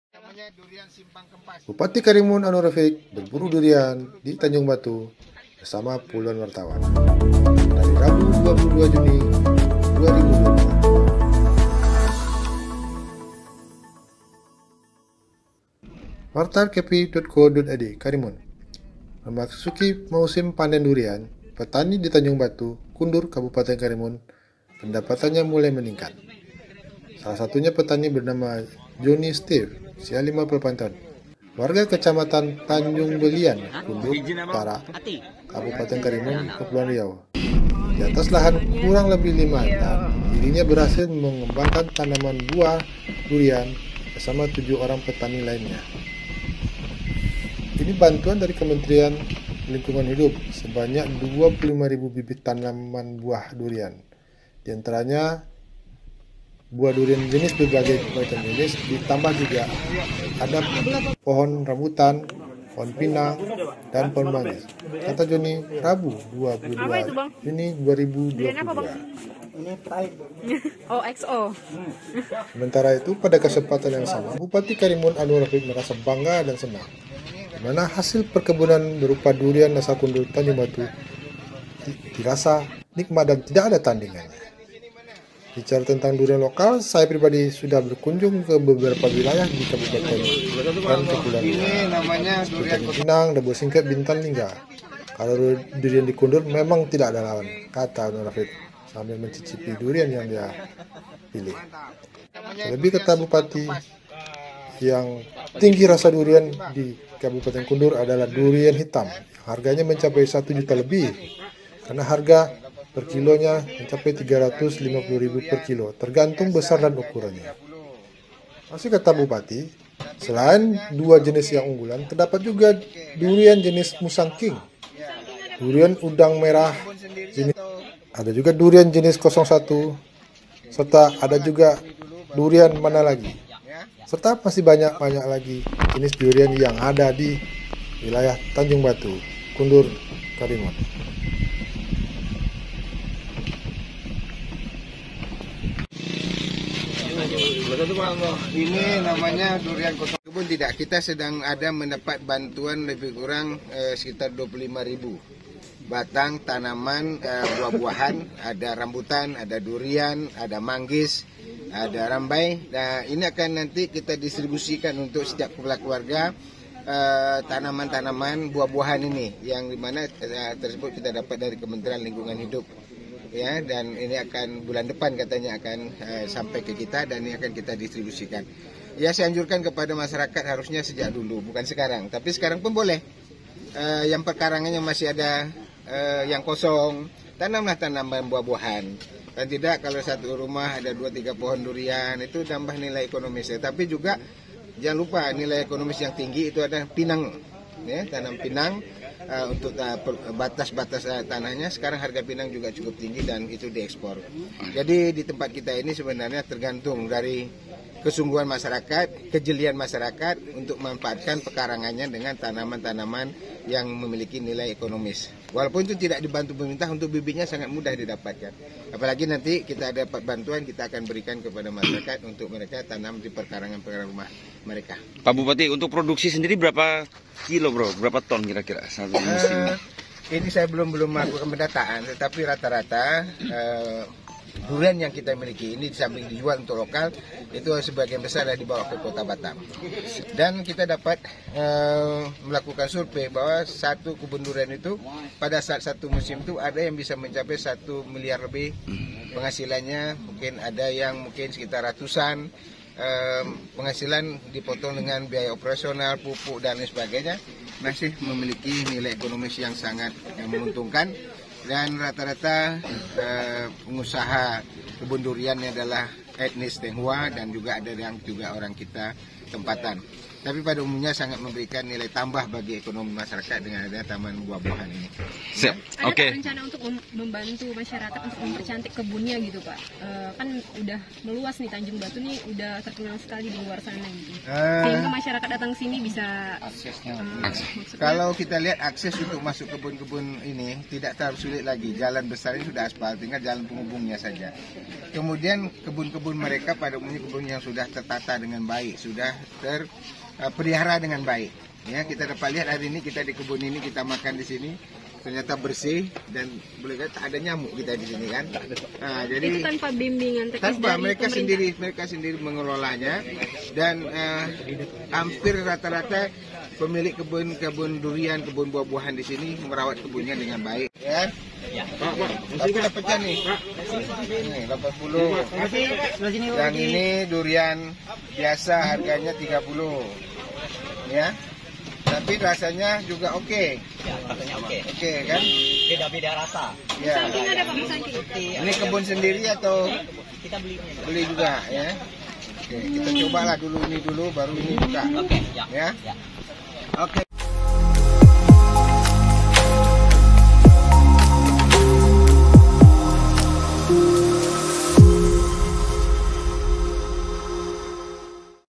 Bupati Karimun Aunur Rafiq Berburu Durian Tanjungbatu bersama Wartawan
WartaKepriTV Bupati Karimun Aunur Rafiq Berburu Durian Tanjungbatu bersama Wartawan

Panen Durian Tanjungbatu, Batam Tujuan Pemasaran, Rafiq: Keuntungan Ratusan Juta lewat @WartaKepri.co.id